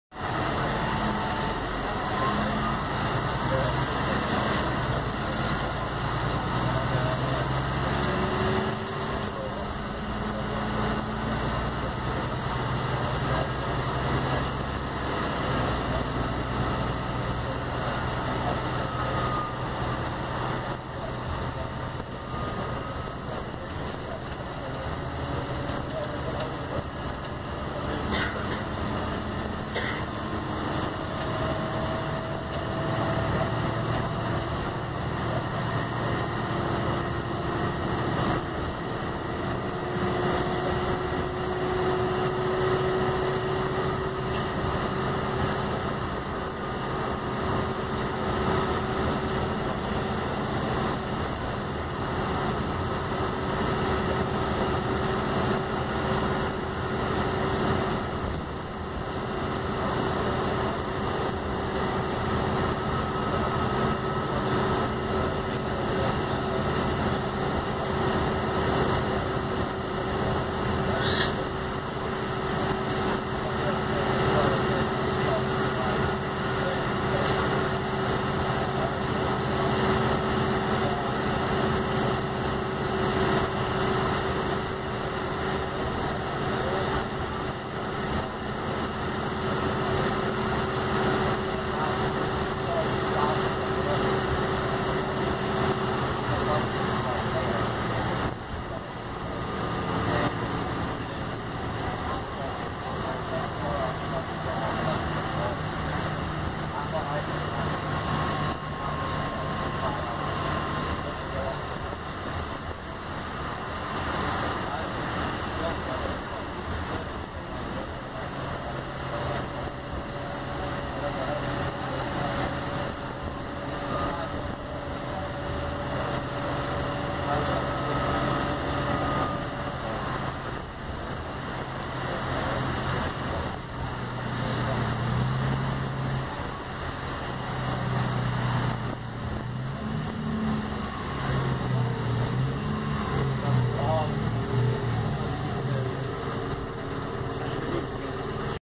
Reliance Buses [BA15 BXS]
Reliance Buses, Euro 6 Volvo B8Rle Wright Eclipse 3 on the 30 to York